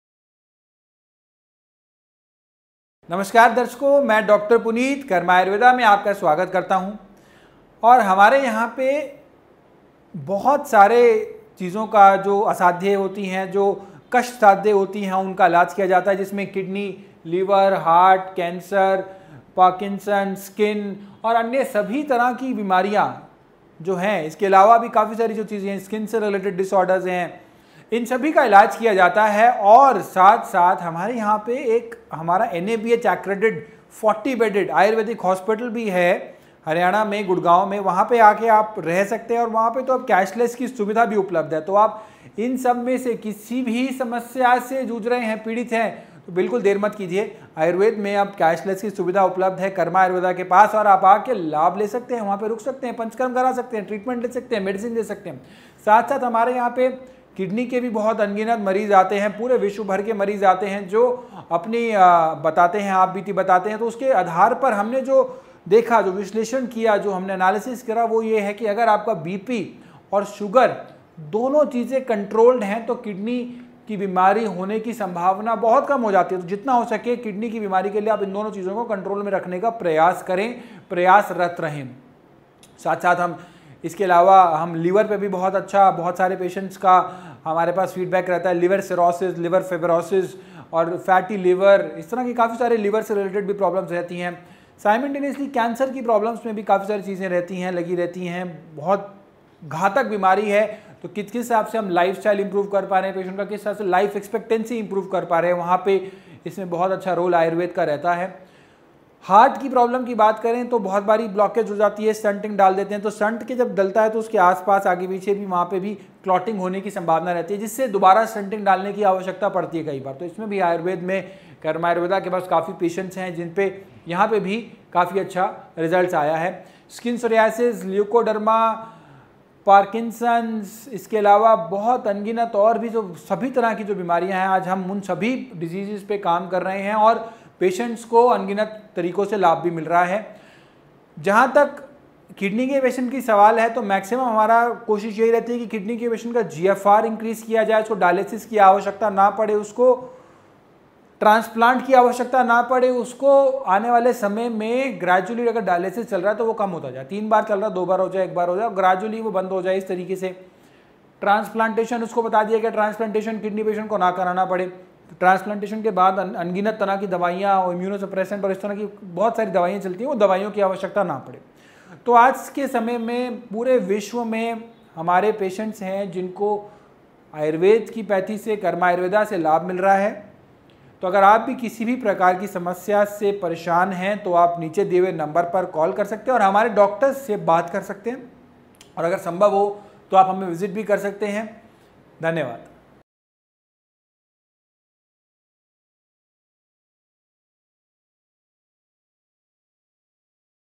कर्मा आयुर्वेदा में मिलेगा हर बीमारी का इलाज | Dr puneet dhawan | karma ayurveda hospital in India
किसी भी बीमारी से सम्बंधित किसी भी जानकारी के लिए हमे सम्पर्क करे ,निचे दिए गए लिंक पर क्लिक करे एंड पेशेंट की Details भरे

Hello, I, Dr. Puneet, welcome you all to Karma Ayurveda. We have treatment for many diseases like liver, kidney, Parkinson's, skin, and all other diseases like skin or liver, heart, and cancer is also treated as well as we have Ayukarma Hospital (NABH Accredited) here in Gurugram, Haryana. Where we also provide a cashless treatment facility. If you are troubled by all these diseases, you can get treatment from our hospital. We also have better treatment for kidneys here. It has been seen that if your blood pressure and diabetes remain under control, then you stay away from kidney disease. Liver cirrhosis, fatty liver, cancer, and other diseases are better treated here. If we talk about the heart, then Ayurvedic treatment has proved very good for the heart. As far as kidney disease is concerned, we try to increase the GFR so that the patient does not need dialysis. In today's time, we have patients all over the world who have been cured by Ayurvedic treatment.